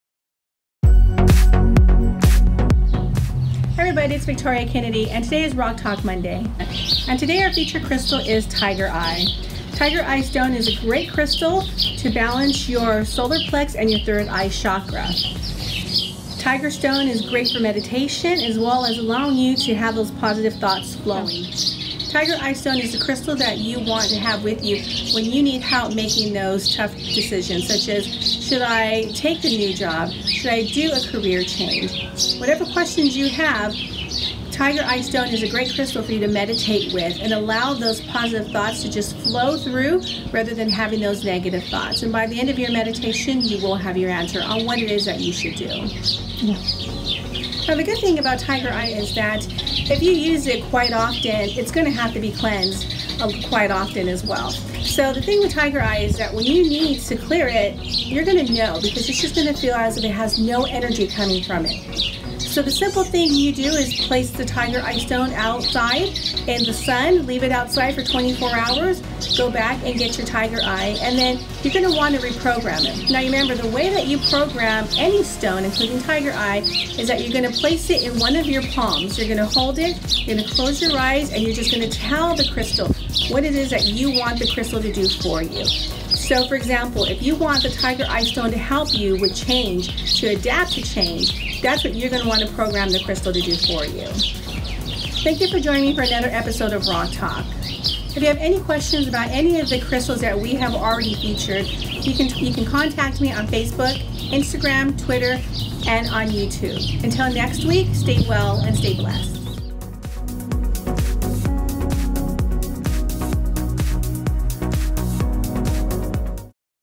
Rock Talk Monday - Tiger Eye Stone
Today we are discussing tigereyestone which is used to clear your solar plexus and third eye chakra. It's great to use for Meditaiton, helping you think positive and with decisions.